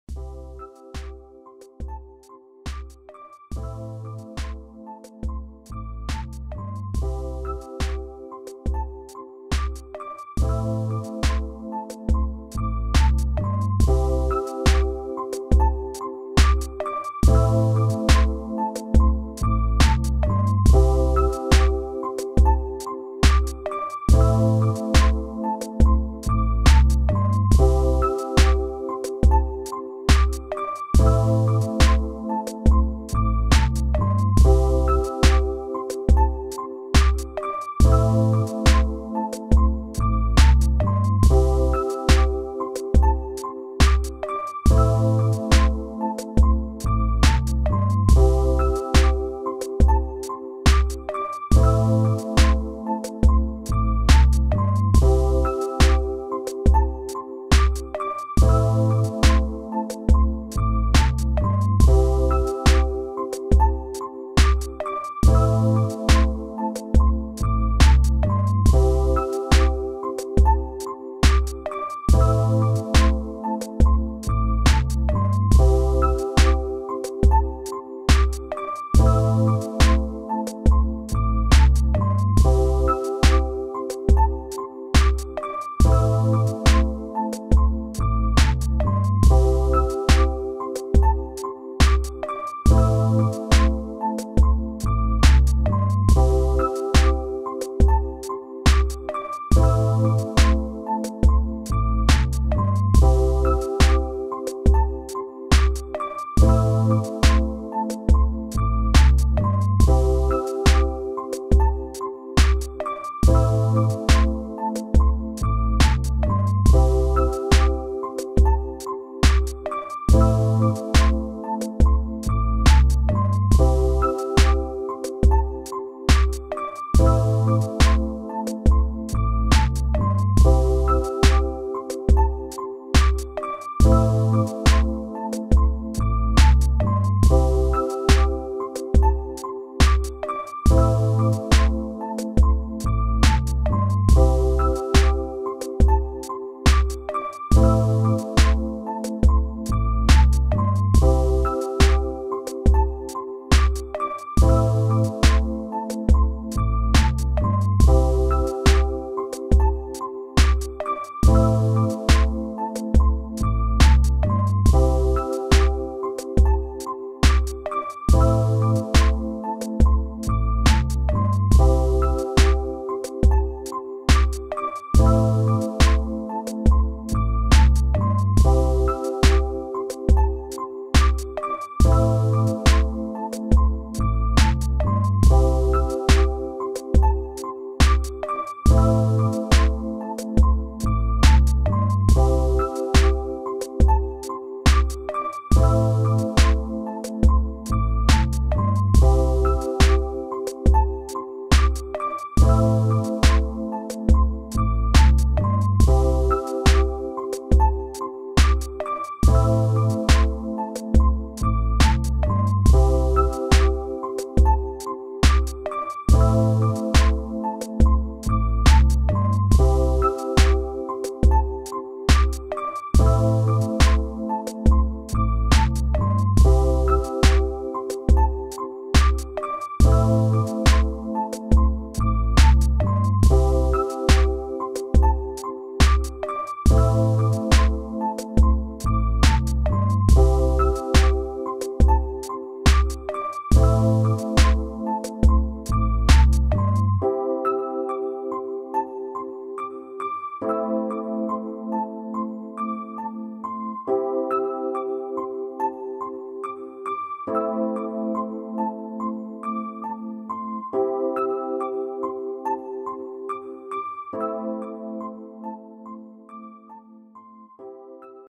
[ENG] HP 오피스젯 8715 유선무한공급기 설치방법!/How to install HP OfficeJet 8715 continuous ink system
❝믿고 구매하셔도 됩니다❞